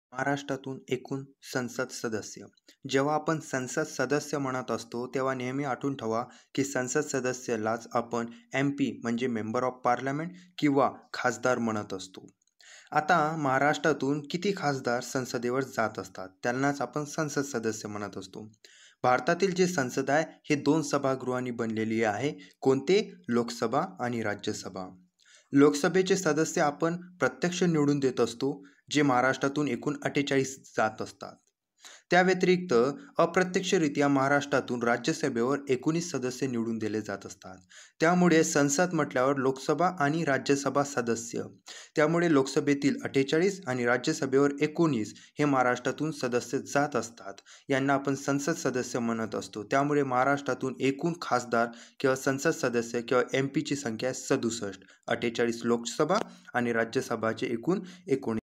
महाराष्ट्रातून एकुण संसद सदस्य
संदर्भ:-
📌लोकसभा राज्यनिहाय जागा :-

✏️जागा:-80
राज्य:-उत्तर प्रदेश

✏️जागा:-48
राज्य:-महाराष्ट्र

✏️जागा:-42
राज्य:-पश्चिम बंगाल

✏️जागा:-40
राज्य:-बिहार

✏️जागा:-39
राज्य:-तामिळनाडू

✏️जागा:-29
राज्य:-मध्य प्रदेश

✏️जागा:-28
राज्य:-कर्नाटक

✏️जागा:-26
राज्य:-गुजरात

✏️जागा:-25
राज्य:-आंध्र प्रदेश,राजस्थान

✏️जागा:-21
राज्य:-ओडीसा

✏️जागा:-20
राज्य:-केरळ

✏️जागा:-17
राज्य:-तेलंगणा

✏️जागा:-14
राज्य:-आसाम व झारखंड

✏️जागा:-13
राज्य:-पंजाब

✏️जागा:-11
राज्य:-छत्तीसगड

✏️जागा:-10
राज्य:-हरियाणा

✏️जागा:-07
राज्य:-दिल्ली

✏️जागा:-05
राज्य:-दिल्ली
✏️जागा:-04
राज्य:-हिमाचल प्रदेश

✏️जागा:-02
राज्य:-अरुणाचल प्रदेश,गोवा,मणिपूर,मेघालय,त्रिपुरा

✏️जागा:-01
राज्य:-मिझोराम,नागालँड,सिक्कीम

All images use in this video are my own.
purpose of this video is only education.